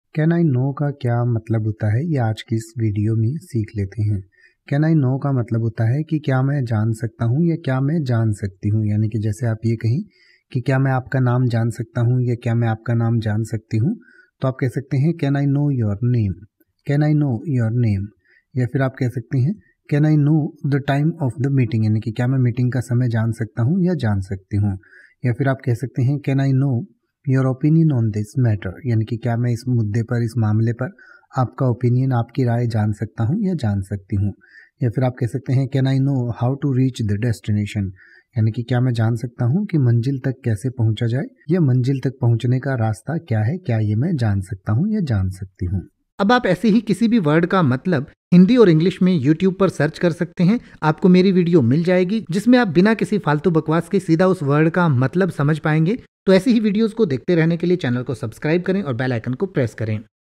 Can i know meaning in Hindi | Can i know ka matlab kya hota hai | English to hindi
इस वीडियो में आप Can i know का हिंदी में मतलब समझेंगे और साथ में Can i know का pronunciation भी सीखेंगें यानि की आप Can i know के अर्थ के साथ साथ ये भी सीखेंगें की Can i know को कैसे बोले या Can i know को बोलने का सही तरीका क्या है |

तो बस एक मिनट में सीखिए Can i know को|
Let’s learn Hindi Meaning of Can i know in detail.

Can i know का हिंदी में अर्थ होता है
क्या मैं जान सकता हूं

Can I know your name?
Can I know the time of the meeting?
Can I know your opinion on this matter?
Can I know how to reach the destination?

Can i know को इन उदाहरणों से और अच्छे से समझ सकते हैं